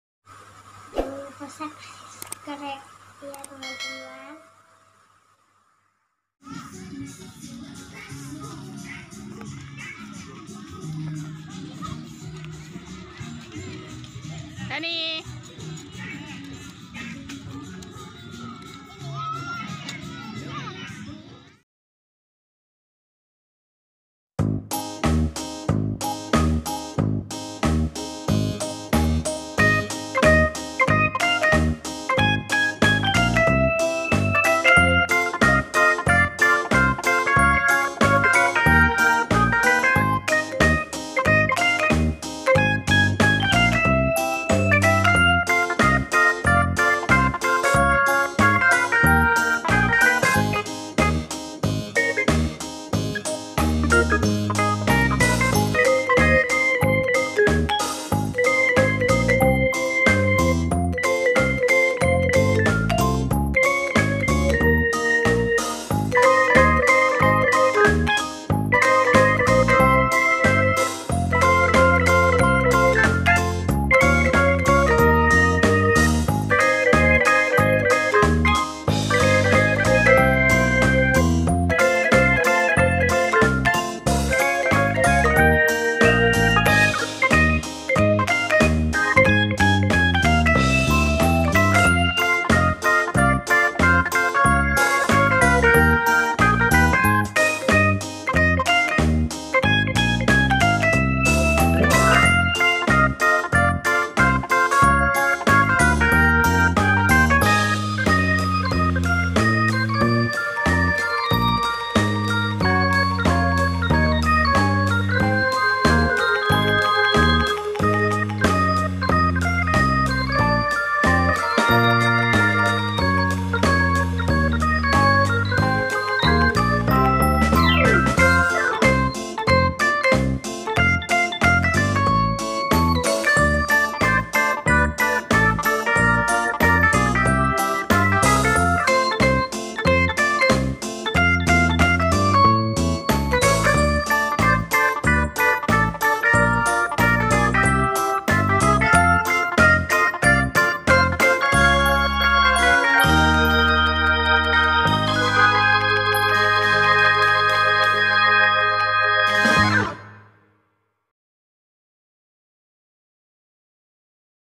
BERMAIN SCOOTER, DI TAMAN KARANG NIO / MUARA AMAN LEBONG
video ini saya rekam langsung ketika Danny Bermain skuter di taman smart city karang nio

Segera kunjungi taman terdekat ini untuk bersantai, olahraga, jogging, jalan santai dan lainnya.